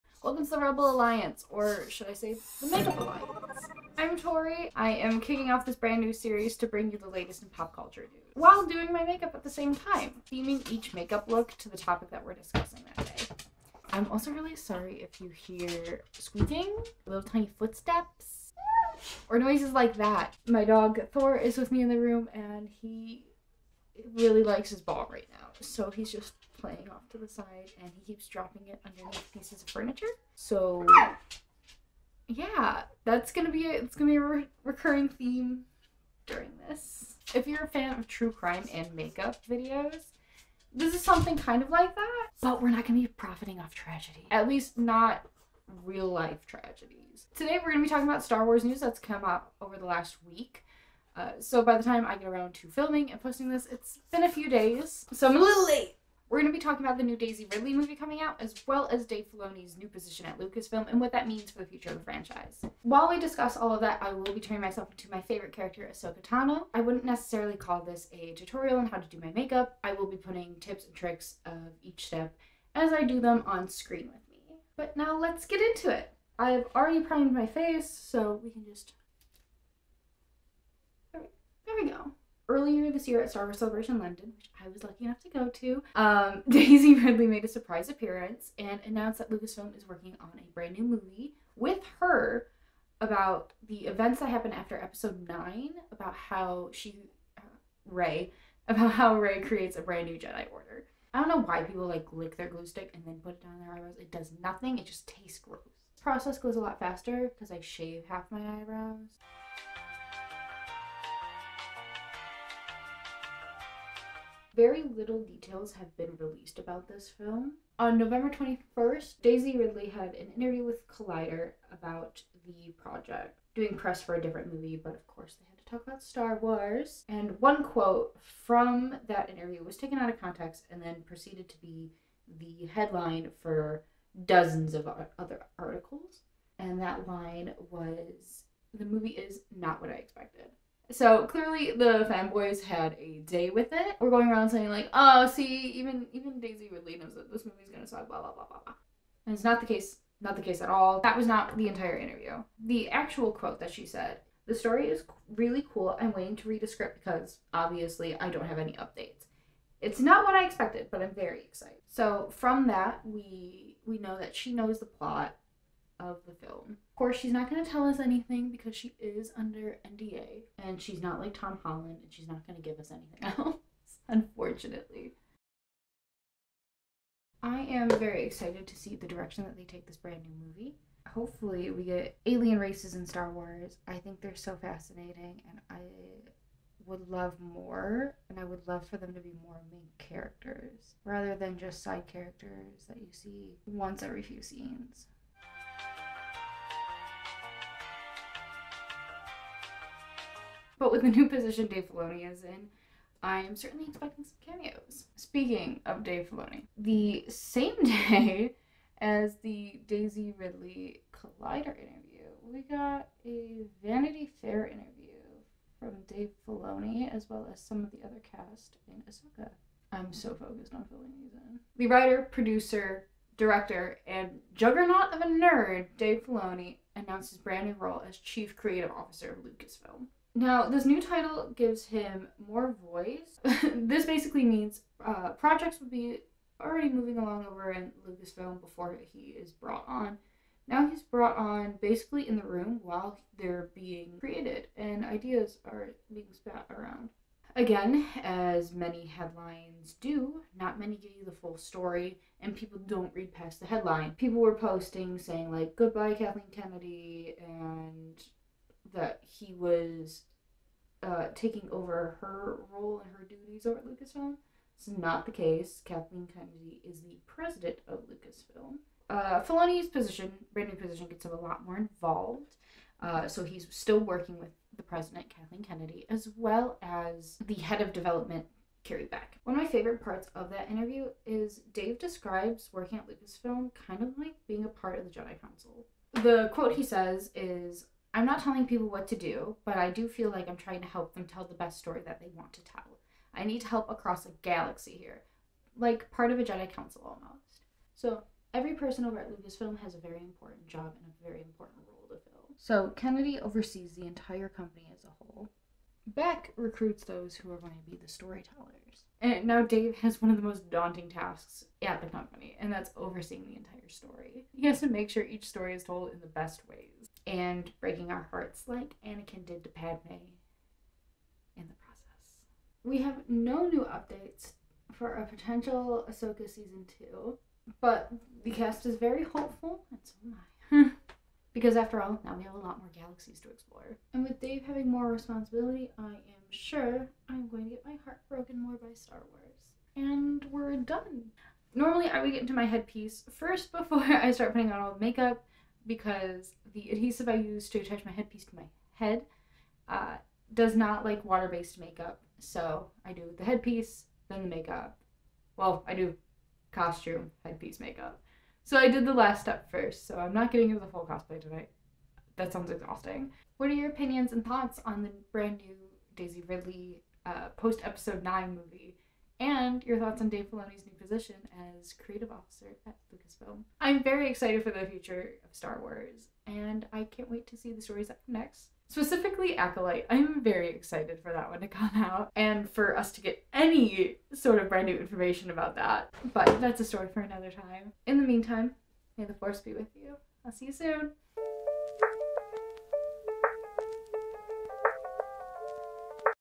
Dave Filoni's Promotion | New Rey Movie | GRWM Ahsoka Makeup
You've heard of the Crime & Makeup videos... Now I bring you a genre for the Star Wars and nerds, Makeup and Pop Culture! I need to think of a catchy name though.

Follow along for a brief tutorial on how I do my Ahsoka Tano Makeup, while we talk about the latest news in the galaxy! LucasFilm's' new Rey movie and Dave Filoni's new job and responsibilities!